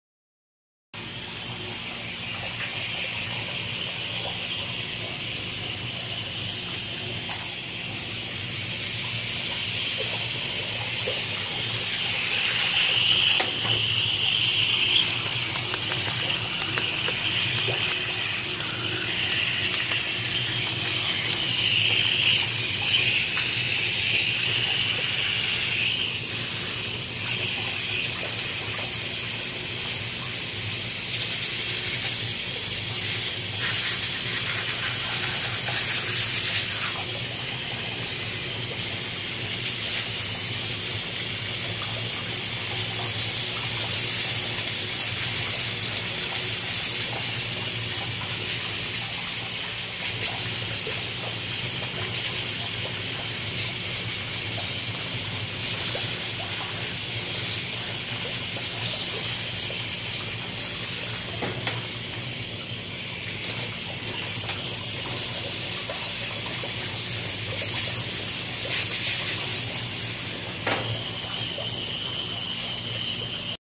台灣祐全洗牛樟木日常
✨目前台灣祐全，已經成功復育牛樟芝的牛樟椴木高達50000棵✨

🎖這就是為什麼，台灣祐全的椴木培養牛樟芝產量，是全台灣最高的！

 💪💪每天小編的同事們，都在細心維護、呵護這些珍貴的牛樟芝！